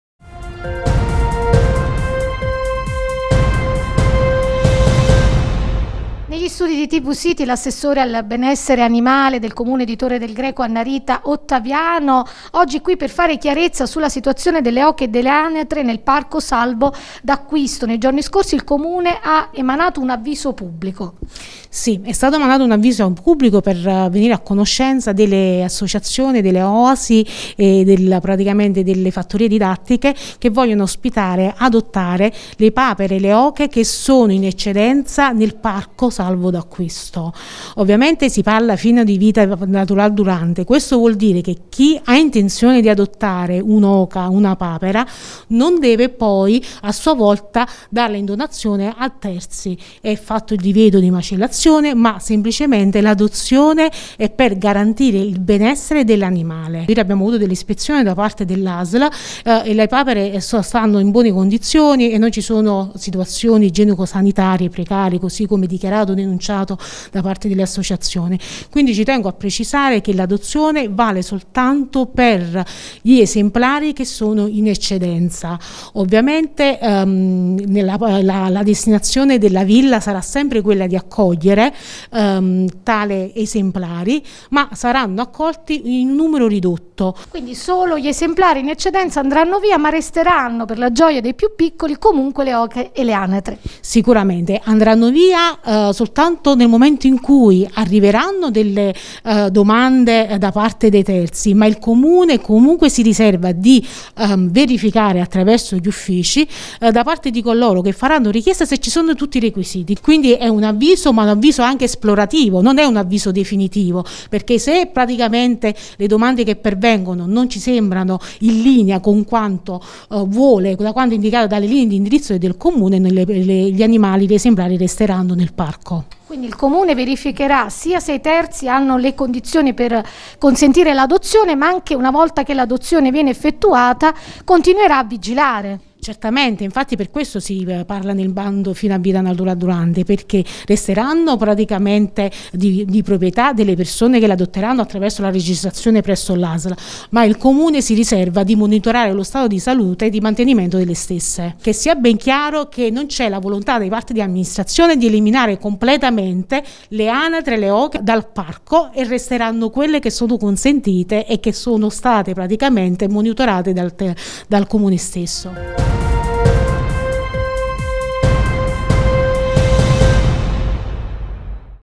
Torre del Greco: al via l'avviso pubblico per adozione oche e anatre Parco D'Acquisto
Ai nostri microfoni, l'assessore al Benessere Animale Annarita Ottaviano fa chiarezza e illustra l'avviso pubblico per l'adozione delle oche e anatre presenti nel Parco "Salvo D'Acquisto"